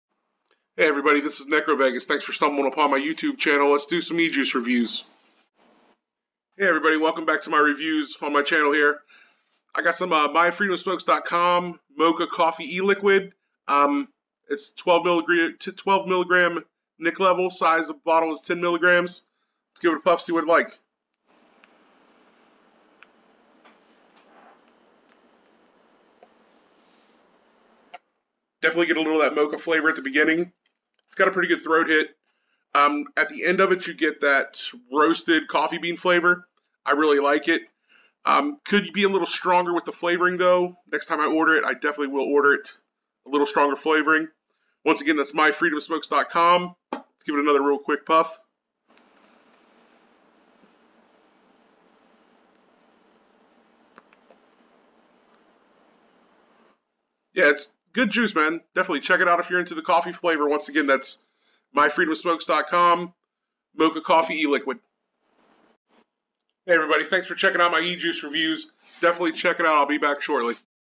myfreedomsmokes.com e-juice review
Just did another review on some vape I got at Myfreedomsmokes.com. Mocha Coffee.  Comments appreciated. Thanks!